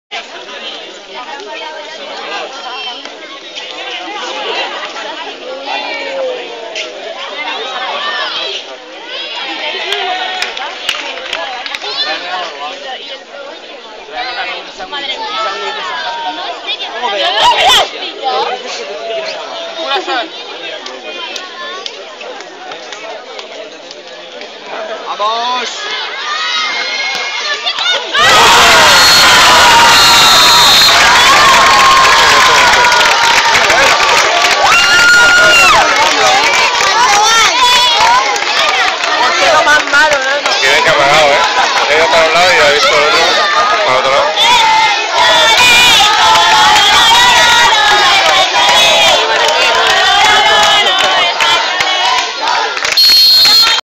Españolators - Ventaquemada. Gol del 3-2
Gol de la final del Campeonato de Futbol Sala de Siete Aguas.
Gol mio contra Ventaquemada, con el que Españolators se proclamaba campeon.